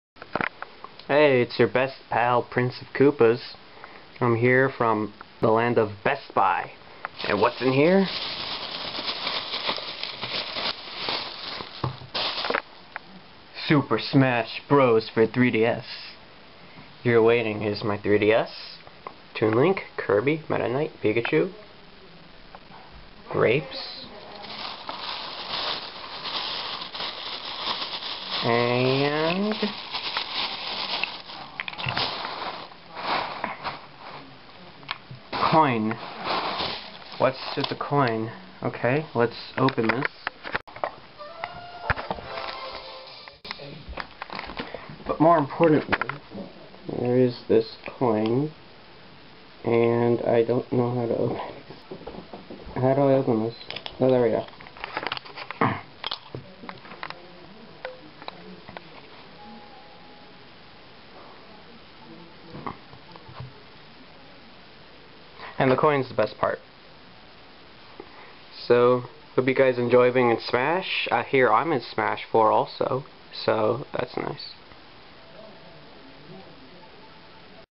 Smash Bros. For 3DS Unboxing... AND GOLD COIN
The Super Smash Bros. For 3DS unboxing...

You can see it's FROM BEST BUY.
YOU KNOW WHAT THAT MEANS.
I GOT THE GOLD COIN HERE.
It's actually pretty weighty.